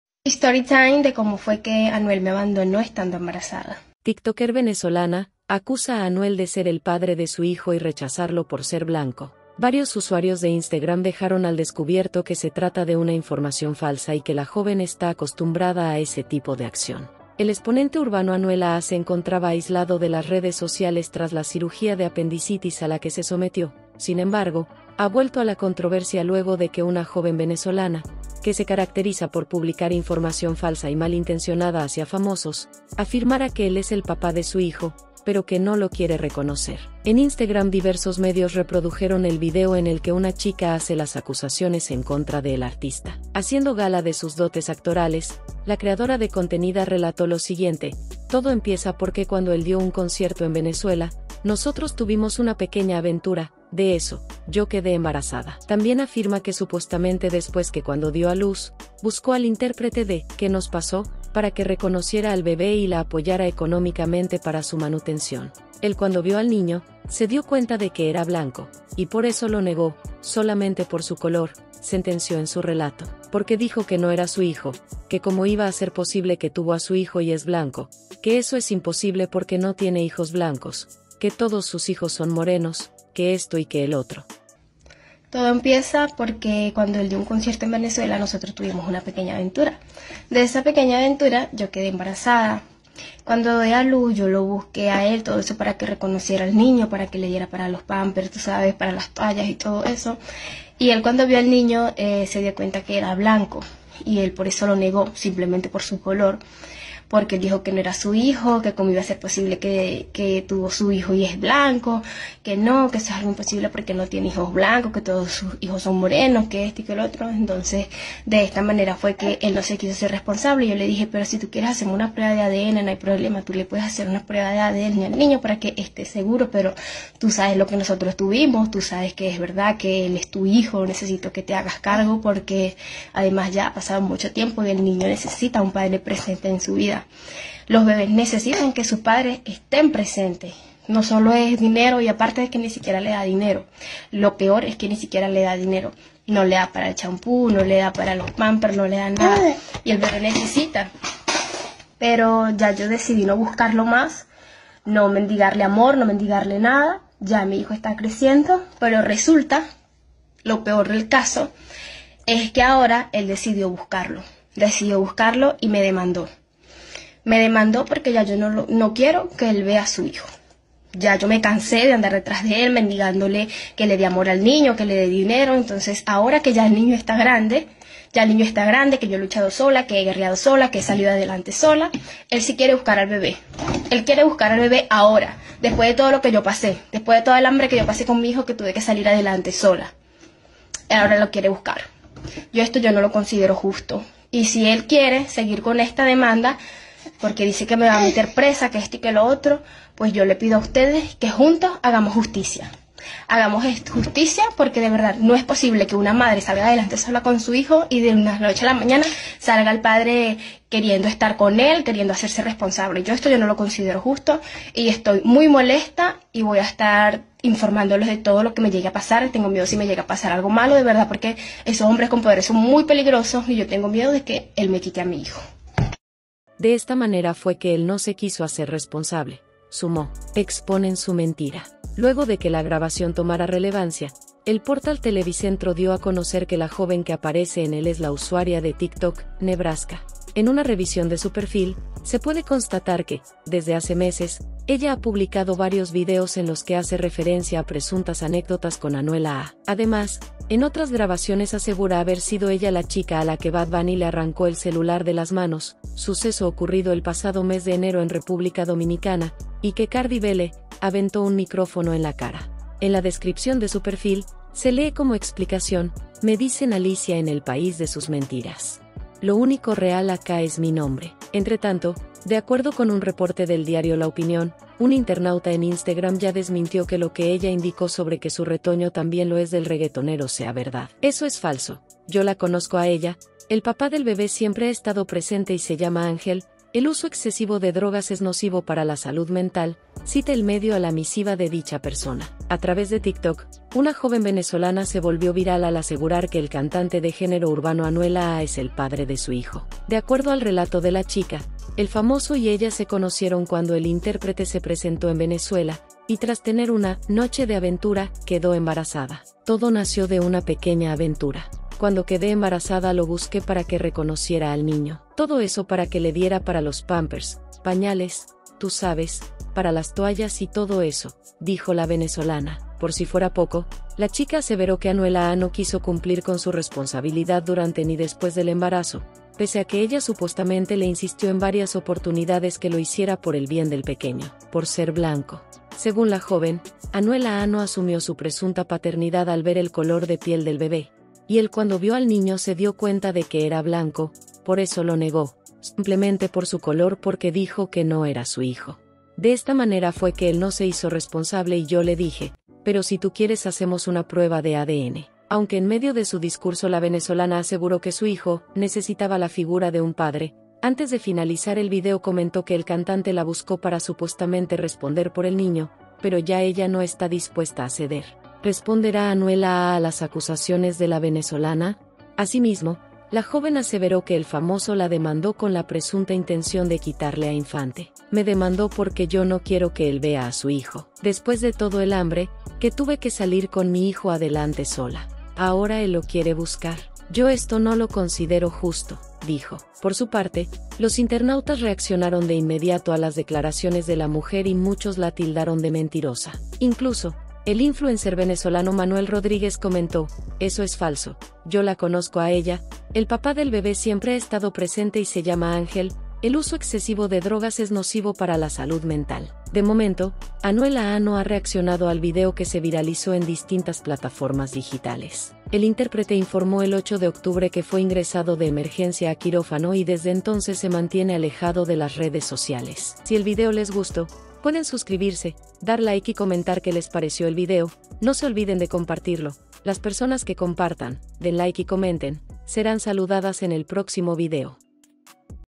TIKTOKER VENEZOLANA ACUSA A ANUEL DE SER EL PADRE DE SU HIJO Y RECHAZARLO "POR SER BLANCO"
Varios usuarios de Instagram dejaron al descubierto que se trata de una información falsa y que la joven está acostumbrada a ese tipo de acción.

El exponente urbano Anuel AA se encontraba aislado de las redes sociales tras la cirugía de apendicitis a la que se sometió, sin embargo, ha vuelto a la controversia luego de que una joven venezolana, que se caracteriza por publicar información falsa y mal intencionada hacia famosos, afirmara que él es el papá de su hijo, pero que no lo quiere reconocer.

En Instagram diversos medios reprodujeron el video en el que una chica hace las acusaciones en contra de el artista.

Haciendo gala de sus dotes actorales, la creadora de contenida relató lo siguiente: "Todo empieza porque cuando él dio un concierto en Venezuela, nosotros tuvimos una pequeña aventura, de eso, yo quedé embarazada".